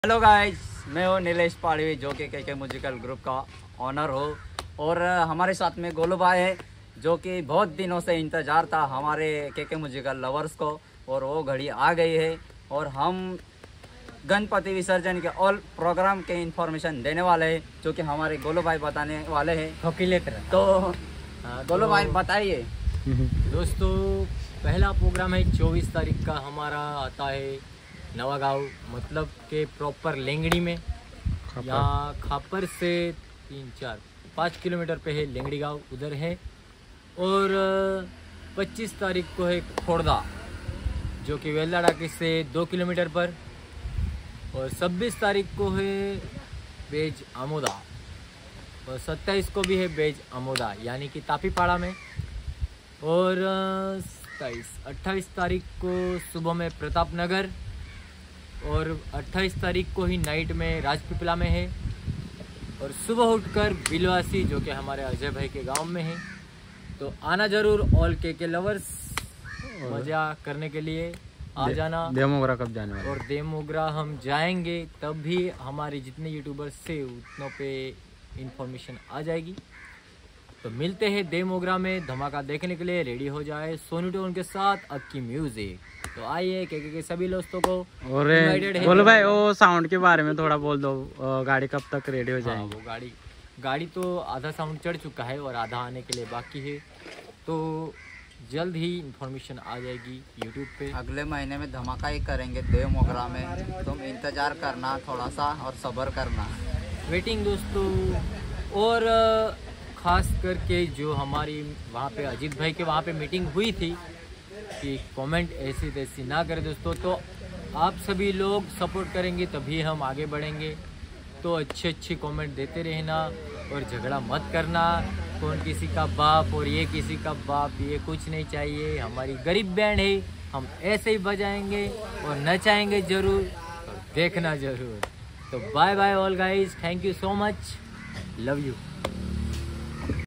KK Musical Information - देव मोगरा कब जायेंगे / गणपती के Program कहा कहा है / और न्यू Sound आएगा💥😱
"This channel shows adivasi culture, Adivasi best band party video and best adivasi song, Adivasi wedding video and Adivasi wedding dance video shows,